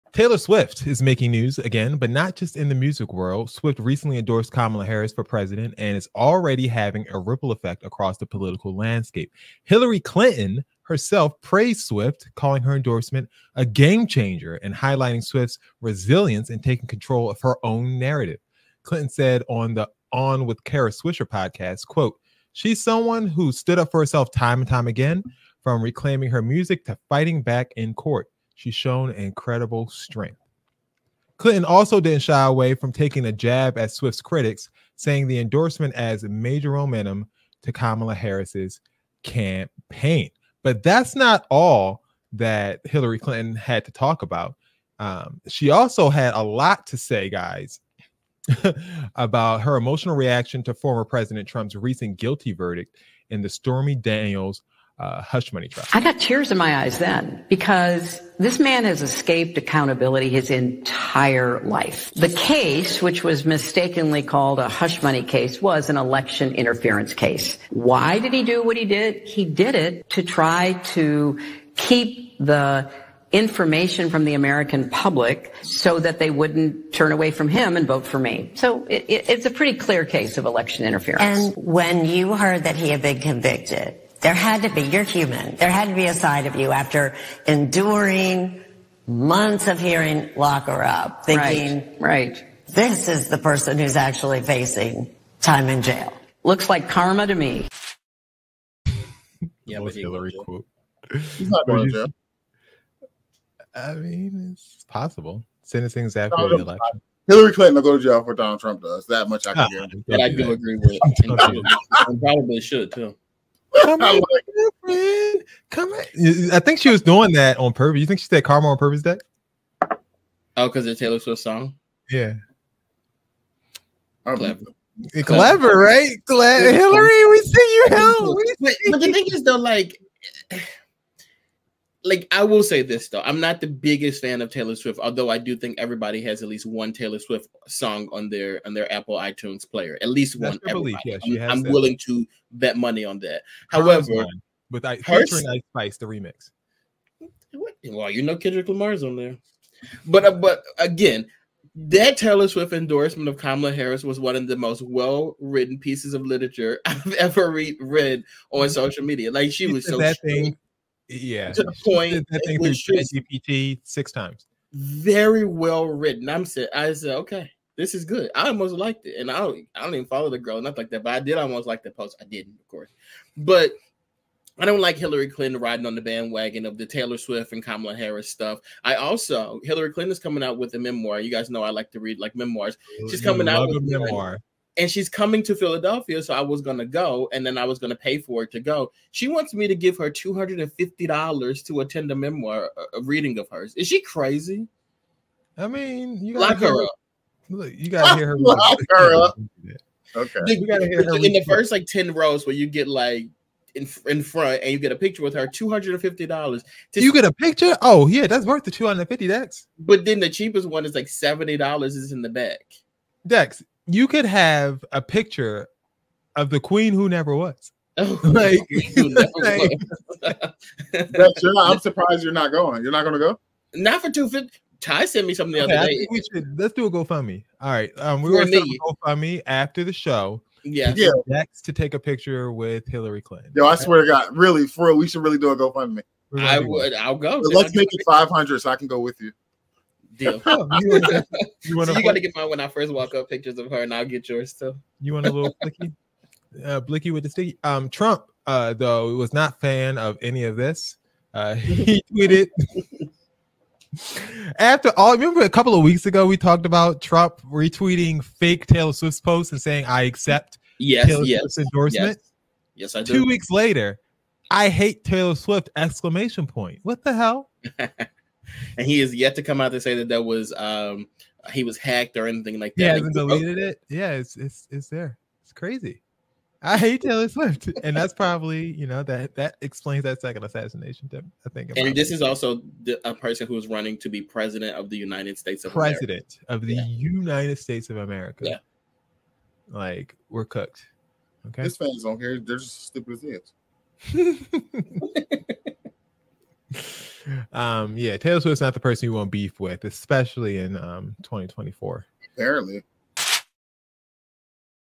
Is Taylor Swift Shaping the 2024 Election? Kamala Harris Endorsement & Hillary Clinton’s Reaction!
Taylor Swift is making headlines, but not just for her music! In this BroTalk LIVE clip, we dive into Swift's bold endorsement of Kamala Harris and its ripple effect across the political landscape. Hillary Clinton calls it a game changer, while Donald Trump definitely has something to say about it. From Swift’s influence to Clinton’s reactions and Trump’s tweets—this is one discussion you don’t want to miss. Politics, pop culture, and controversy all collide here! 🎤🎶🔥